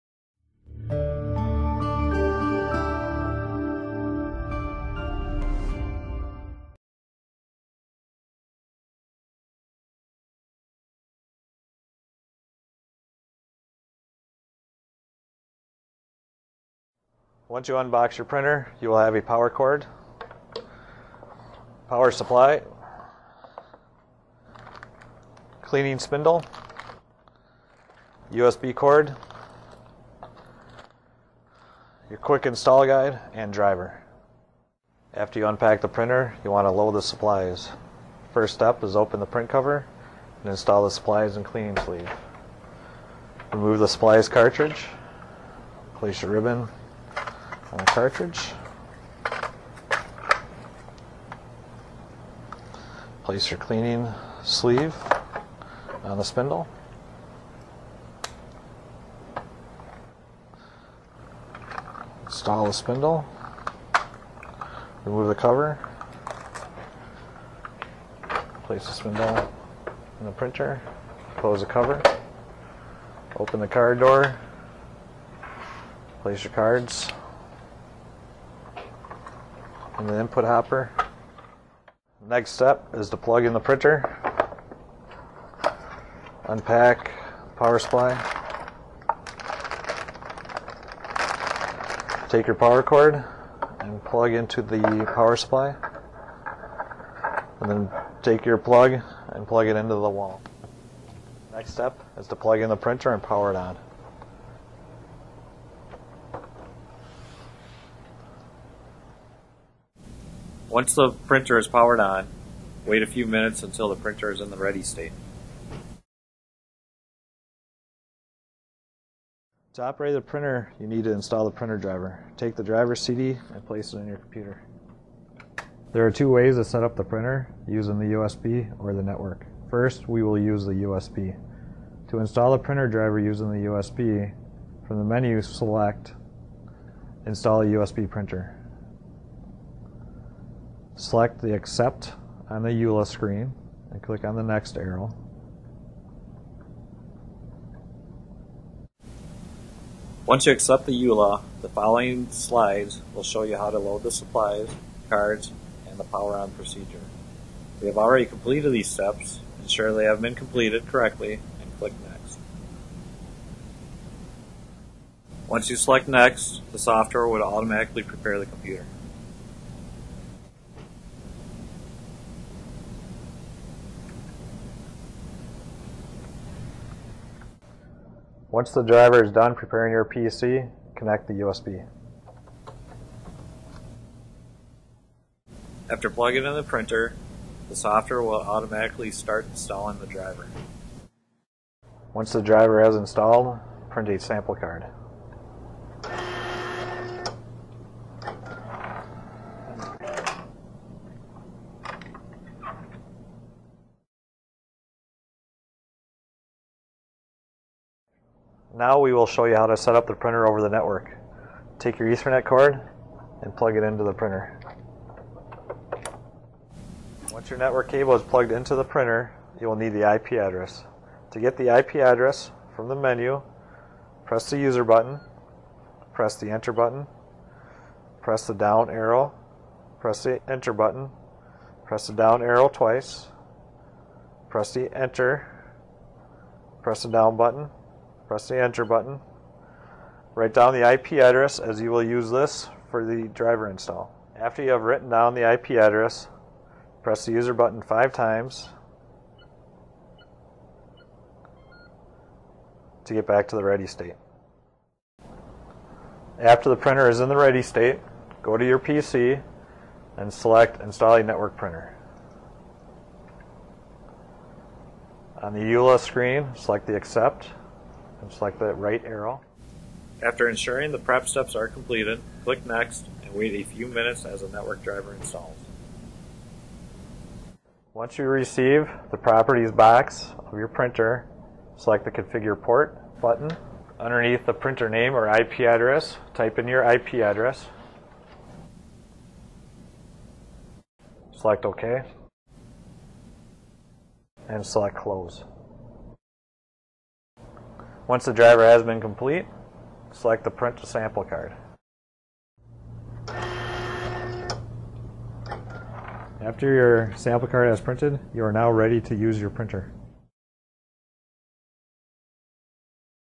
Datacard SD und CD Kartendrucker - Installationsanleitung
Wie installiert man einen Datacard SD oder CD Kartendrucker? In diesem Video erfahren Sie alle Schritte die zur Inbetriebnahme Ihres Kartendruckers nötig sind. Vom Auspacken, über das Installieren der Treiber, die Konfiguration im Netzwerk und Installation der Software wird alles Schritt-für-Schritt erklärt.

Für weitere Informationen stehen wir Ihnen natürlich jederzeit zur Verfügung.